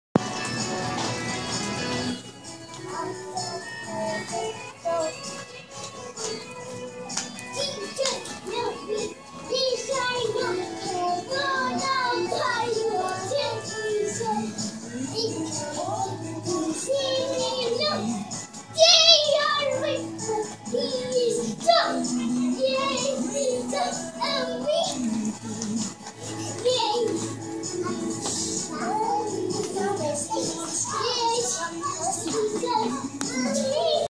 It's Time To Dance
Sera and Sammy do some dancing at Southpark Meadows.